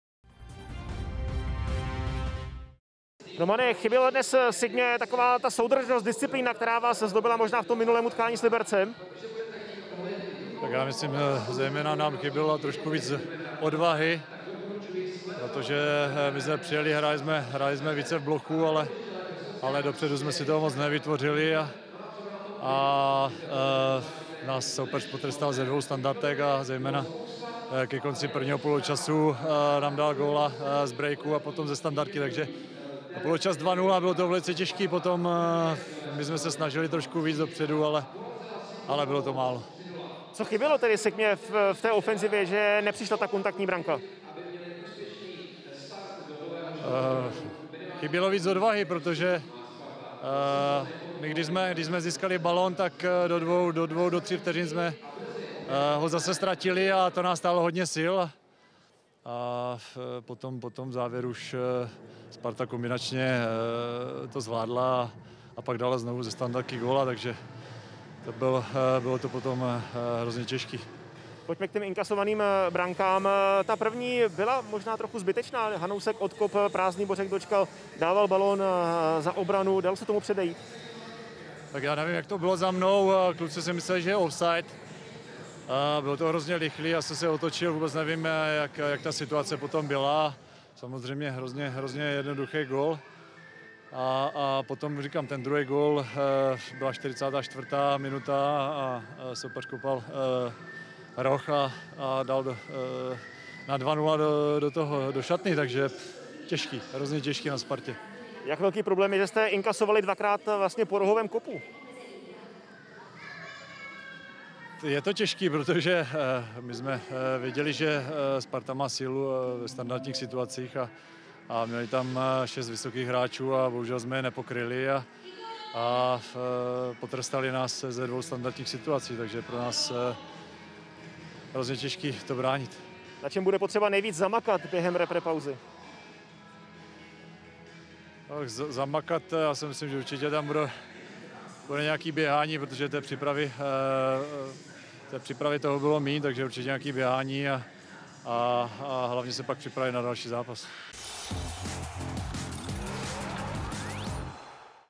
Olomoucký Hubník o prohře na Spartě: Chybělo nám více odvahy
"Chybělo nám trošku víc odvahy, hráli jsme v bloku, ale dopředu jsme si toho moc nevytvořili. Soupeř nás potrestal ze dvou standardek, poločas 0:2 a už to bylo těžké. Snažili jsme se hrát více dopředu, ale bylo to málo," přiznal sebekriticky.